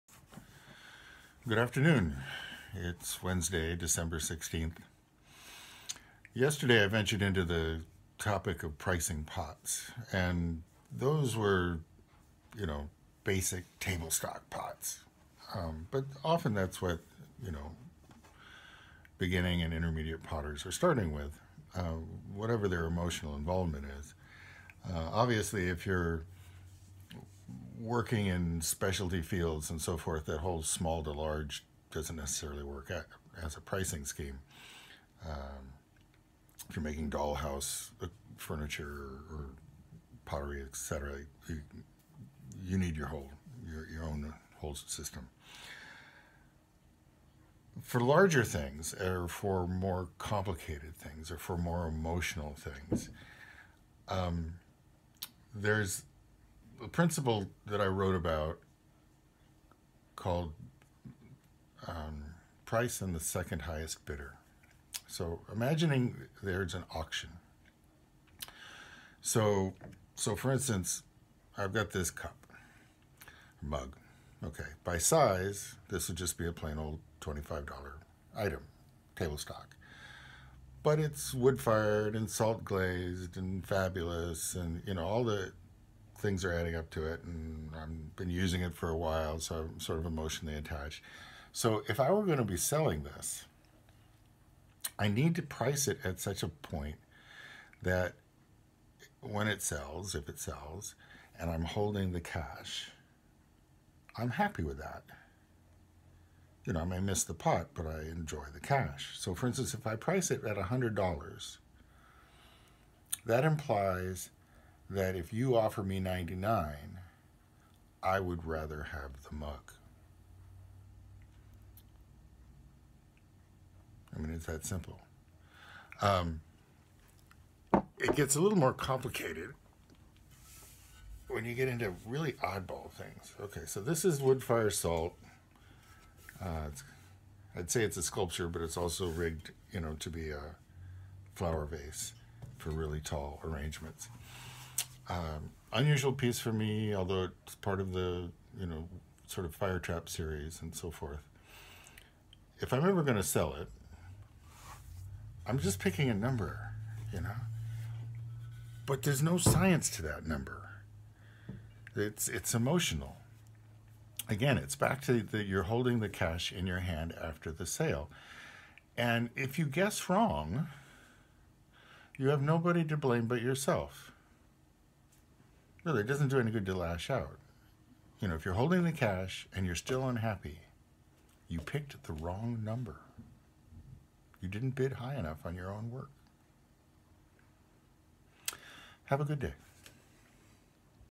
Price and the Second Highest Bidder December 16 2020 IMG 5694
Learning to price things so that when they sell, if they sell, you aren't disappointed about the money. YOU decide how much money is "enough." Don't set the price too low. When the customer leaves with the pot, it's up to you to be happy with the money.